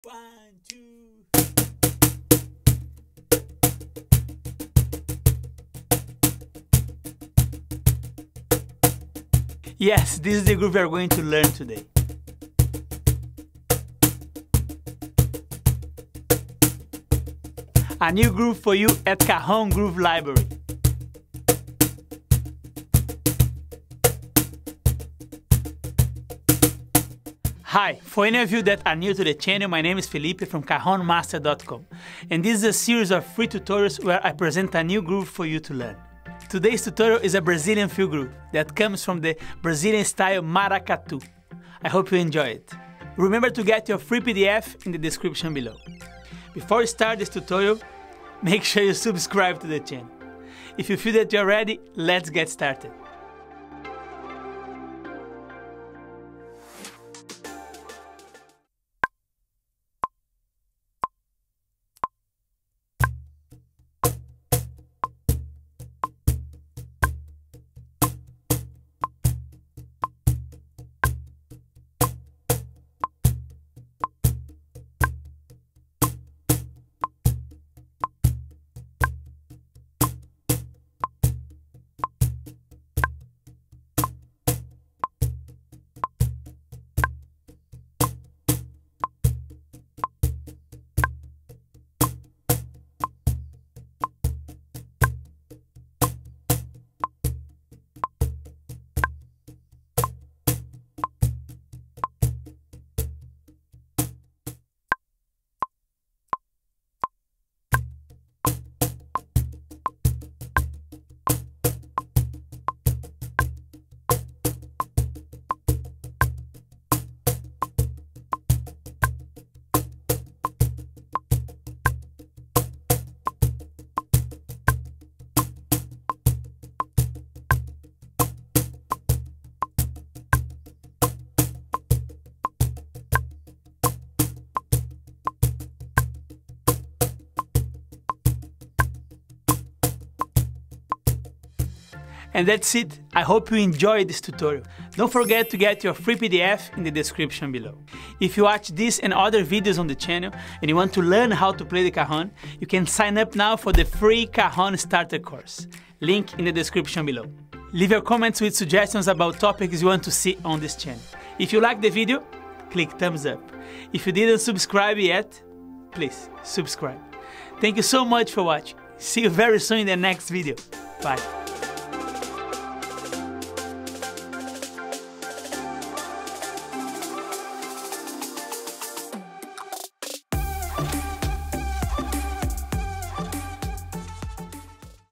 CAJON GROOVE LIBRARY - Brazilian Feel groove #2
In this video tutorial you will learn how to play another Brazilian groove that comes from the Northeastern Brazil. This is just variation from the Maracatu style I’ve simplified and adapted to play on the Cajon. I hope you can take it as an idea for other grooves and also to create some Cajon fills. I’ve included a song list in the Free PDF for you to practice the groove.

More FREE Tutorials!

If you want to learn how to play the Cajon from the very beginning, you can signup now for the